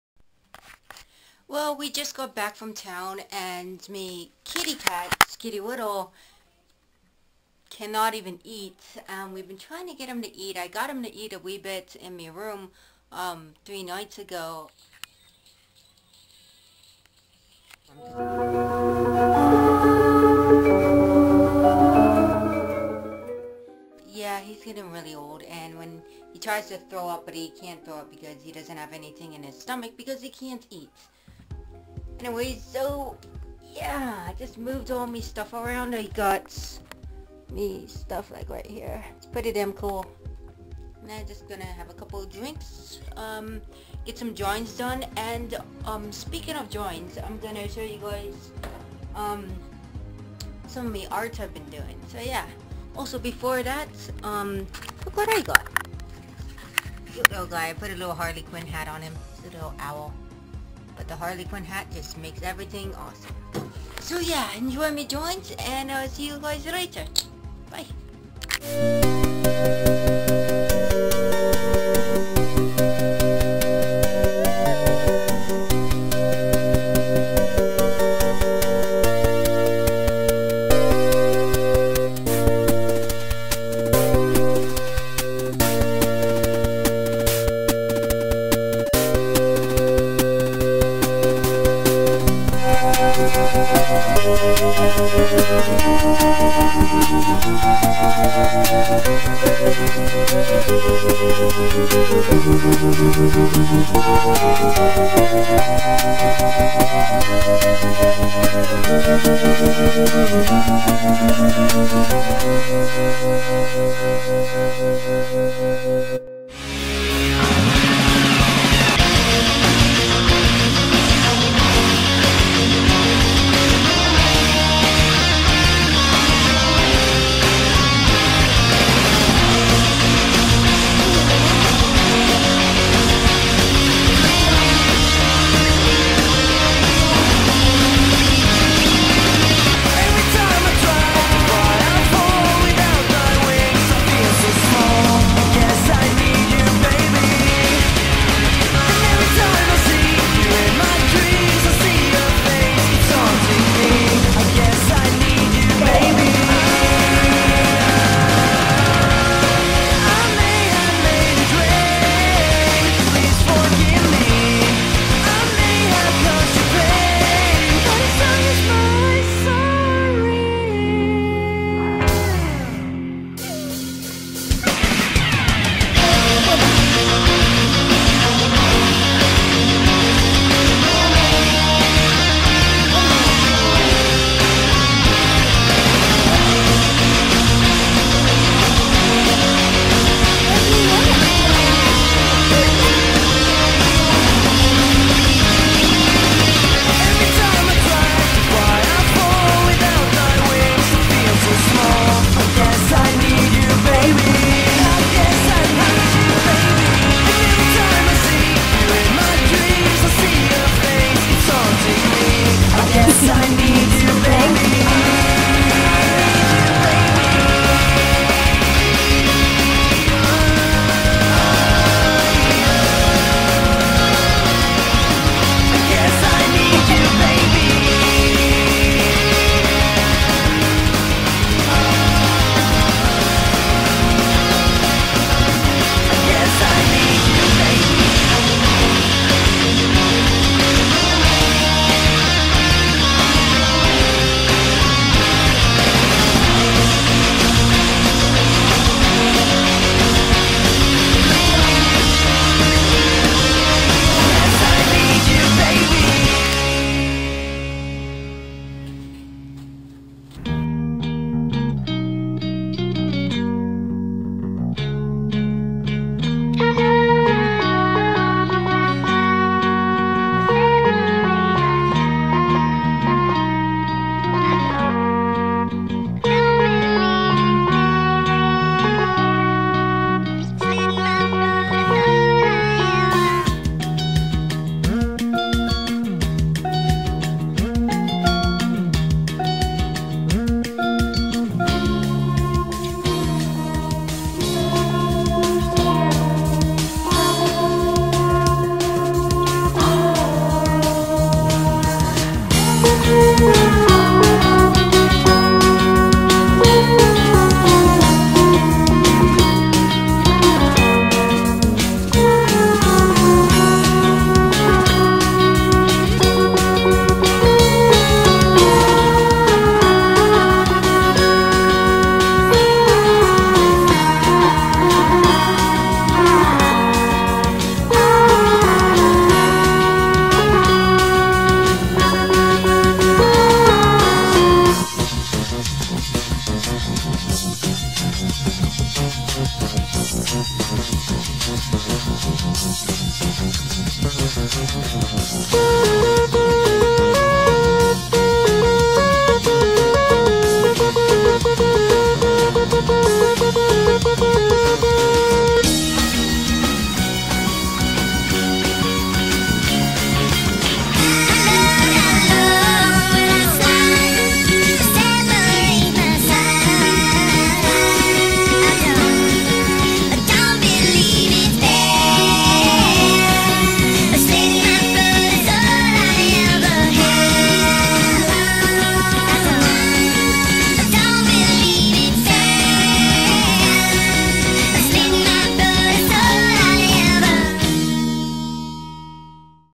gothamcat vlogs - my art, and a spider in my hand
i wasn't in any danger.
iv played with them for along time and they are fun :)
afterwards i fed it to me kitten Fizzy :)